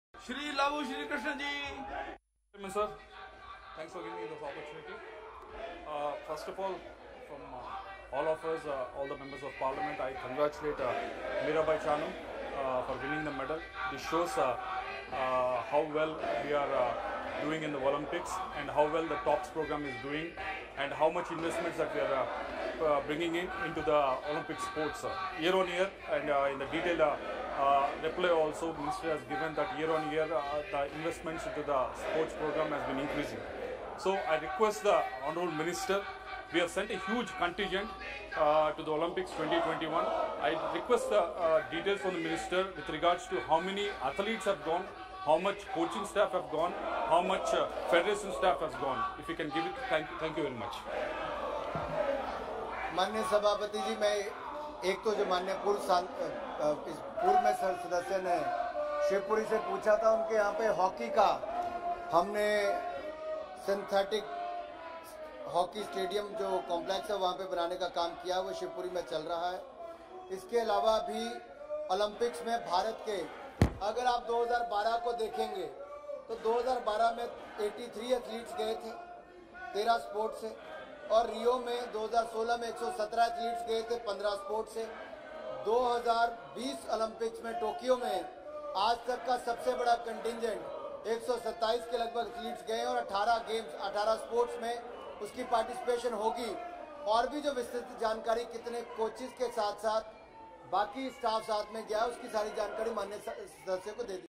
YSRCP MP Lavu Sri Krishna Devarayalu questions Central Govt on National Sports Policy
Welcome to CSNC Media
CSNC Media does not own the rights to these video clips.
▬▬▬▬▬▬▬▬▬▬ஜCSNC MEDIAஜ▬▬▬▬▬▬▬▬▬▬▬

▬▬▬▬▬▬▬▬▬▬▬ஜ۩۞۩ஜ▬▬▬▬▬▬▬▬▬▬▬▬▬
⏬ DOWNLOAD !